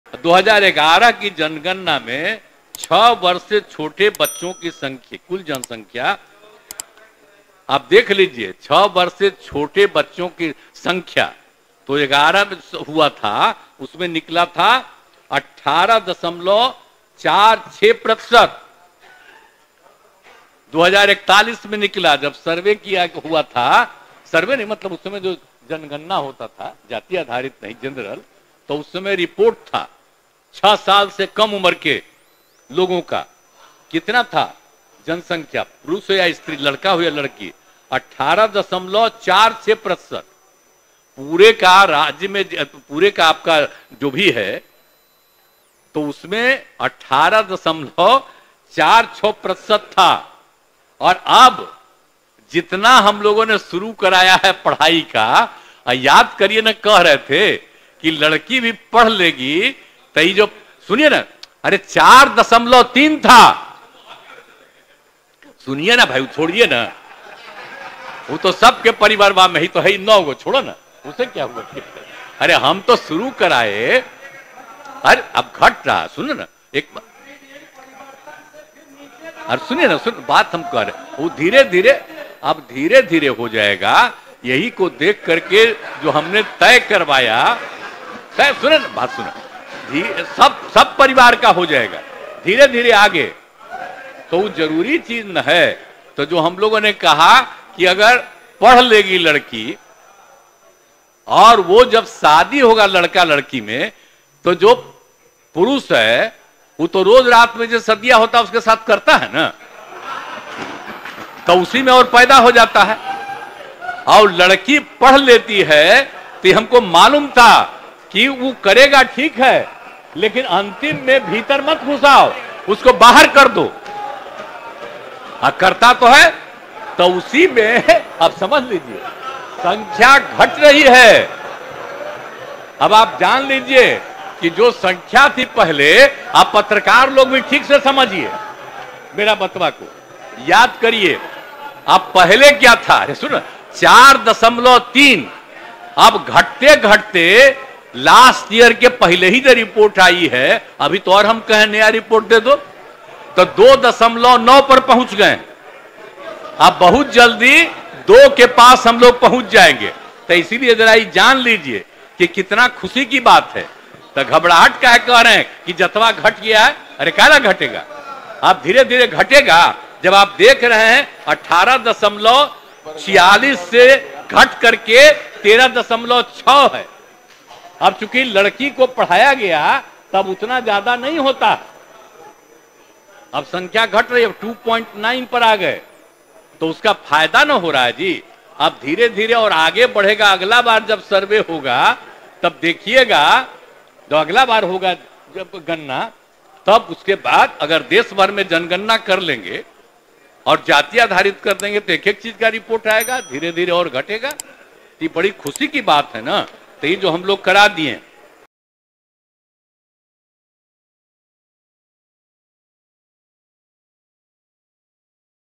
Bihar Caste Survey Report पर बहस के दौरान Nitish Kumar महिलाओं के लिए क्या बोल गए! Viral Video
बिहार जाति आधारित सर्वे के आंकड़ों पर विधानसभा में चर्चा के दौरान सीएम नीतीश कुमार ने असहज करने वाला बयान दिया है। जनसंख्या नियंत्रण और महिला शिक्षा के मुद्दे पर बोल रहे सीएम नीतीश ने ऐसा बयान दे दिया, कि   विधानसभा के अंदर विधायक भी असहज दिखाई दिए।